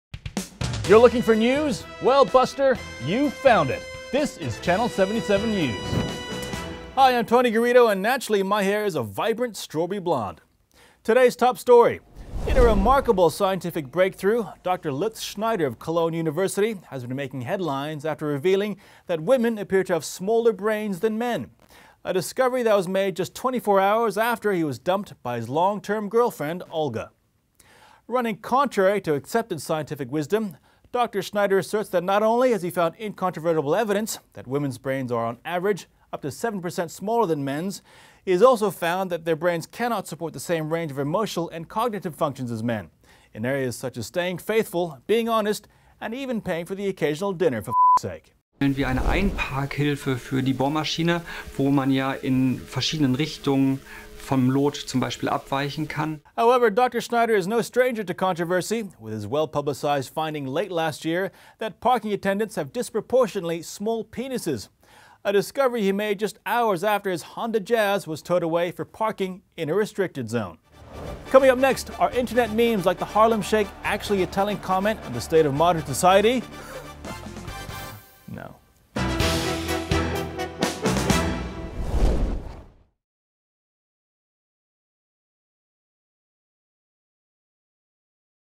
Jilted Scientist Discovers 'Women Have Smaller Brains'
In a remarkable scientific breakthrough, Dr Lutz Schneider of Cologne University has discovered that women have smaller brains than men - a discovery made just after he was dumped by his long-term girlfriend, Olga.